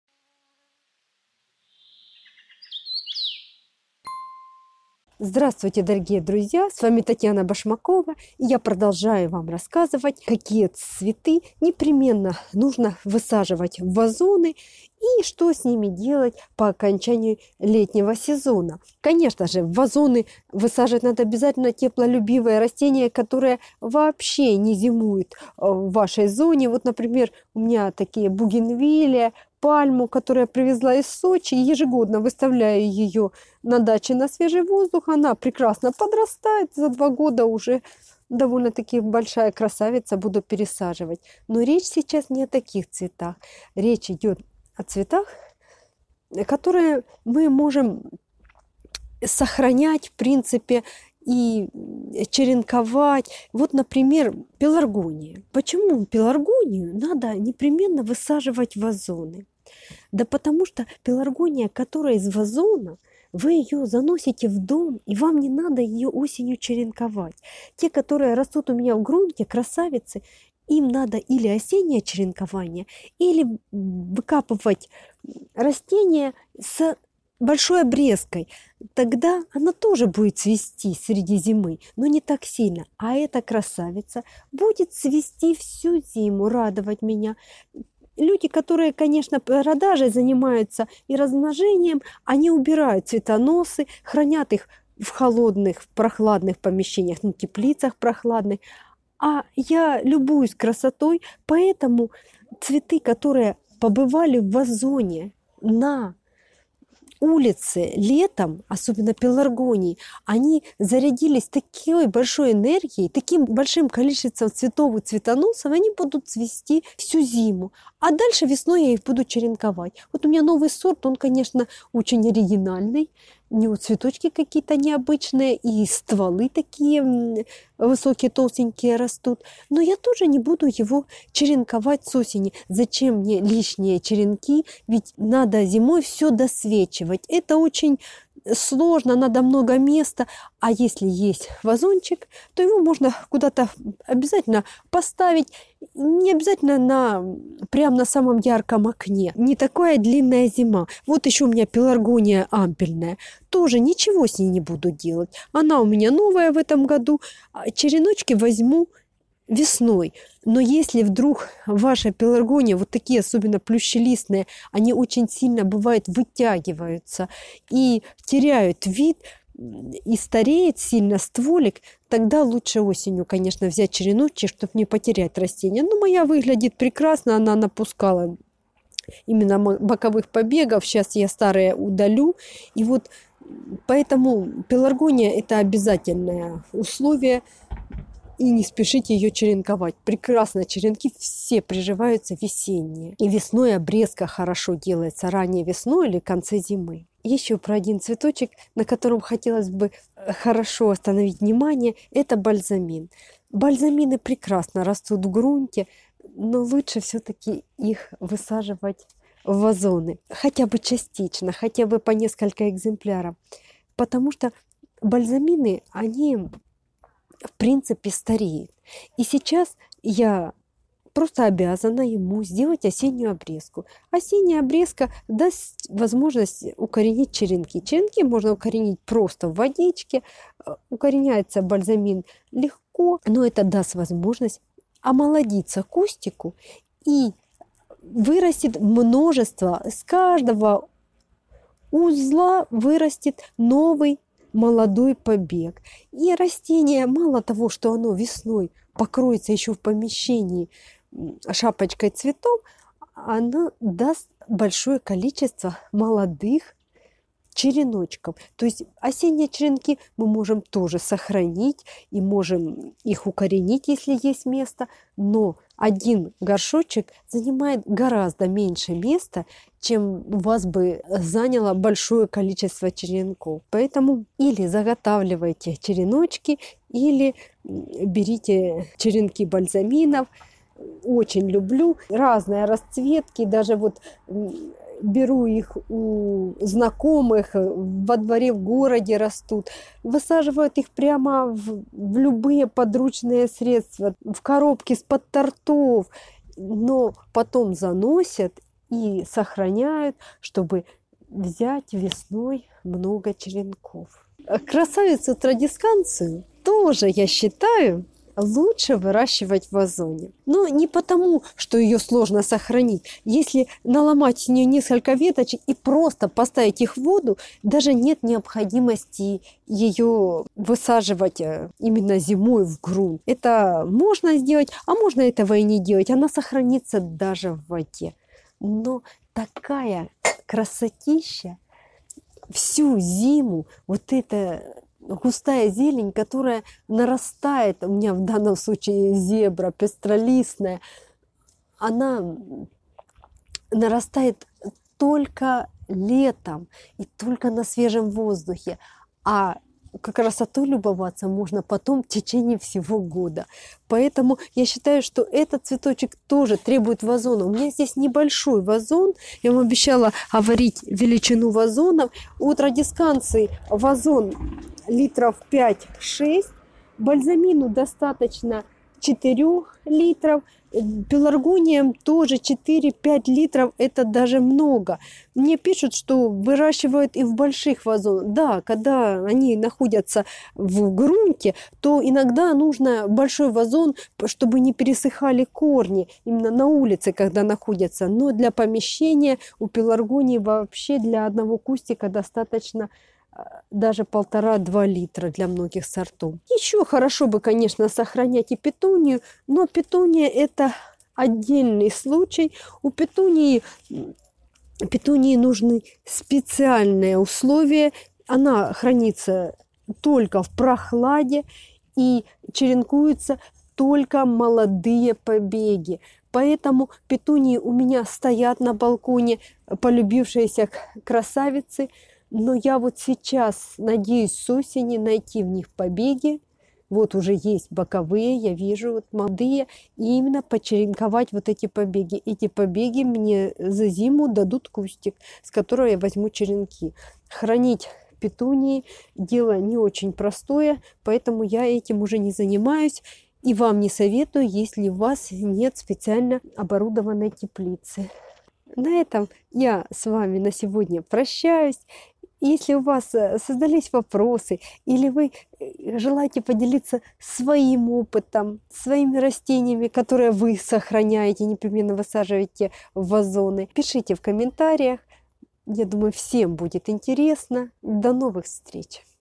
ЧТО ДЕЛАТЬ ПЕРЕД ЗИМОЙ? ЦВЕТЫ В ВАЗОНАХ! часть 2 Пеларгонии, бальзамины, традесканции
Много растений просто необходимо выращивать в кашпо, горшках, вазонах, чтобы хорошо сохранить зимой и легко размножить весной. Таким образом мы используя минимум места, окружая себя цветущей красотой всю зиму, можем сохранить и приумножить пеларгонии, бальзамины, традесканции и мн. др.